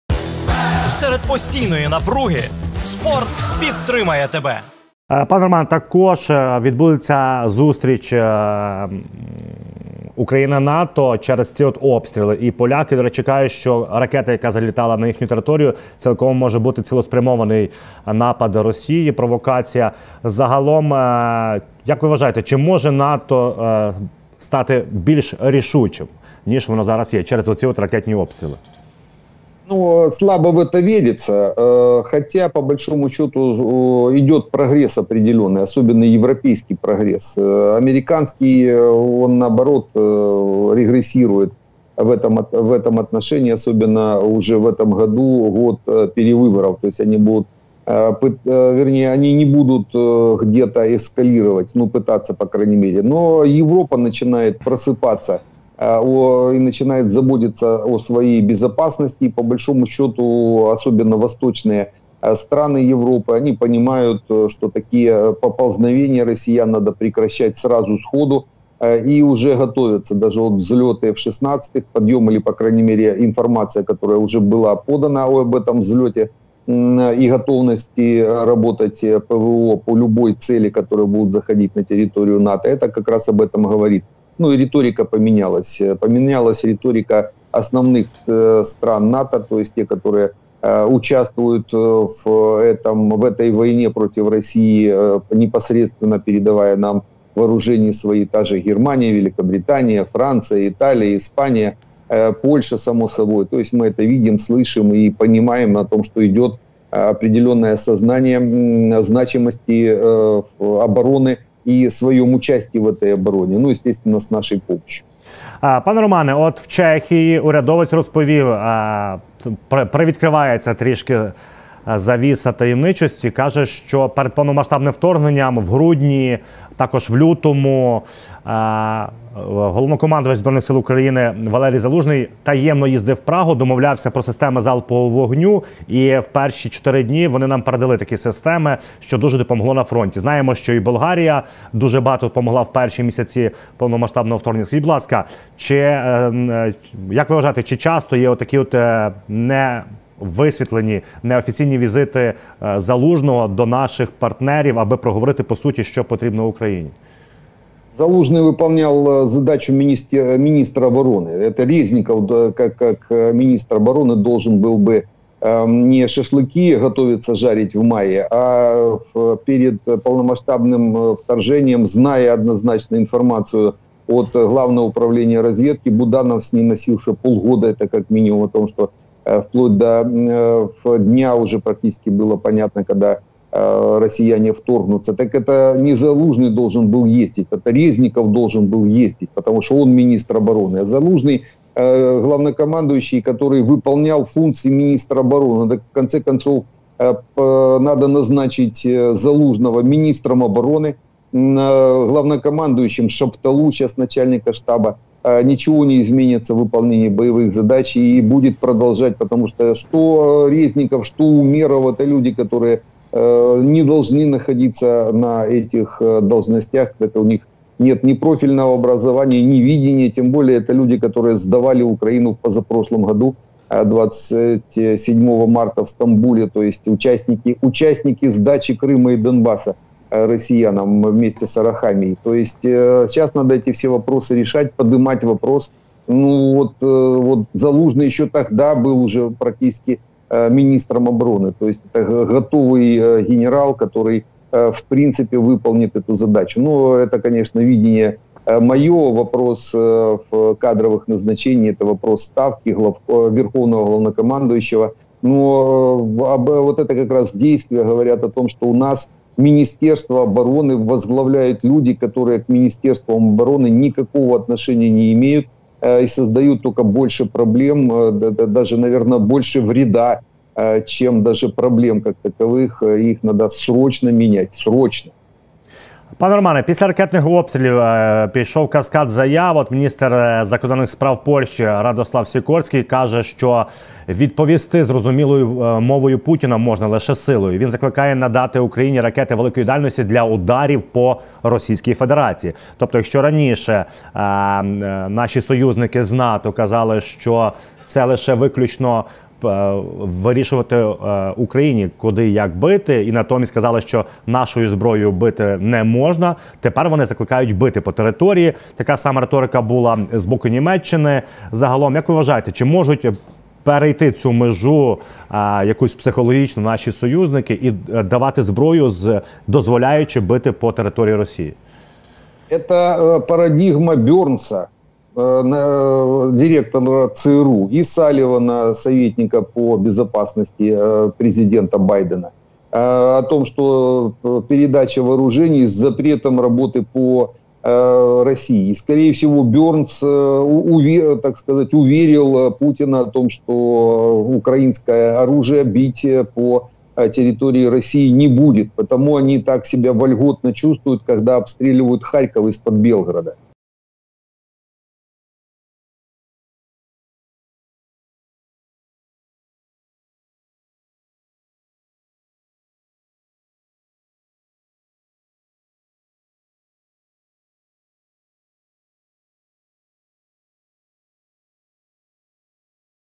СВІТАН: Європа ПРОКИДАЄТЬСЯ: СТРАХ змушує ДІЯТИ / Провокація РФ / ЗАЛУЖНИЙ новий міністр ОБОРОНИ?
Захід нарешті прокидається і починає діяти. Полковник ЗСУ у запасі, військовий експерт, льотчик-інструктор Роман Світан розповів, що у стратегії західних союзників щодо війни відбувається кардинальне зрушення у підтримці України.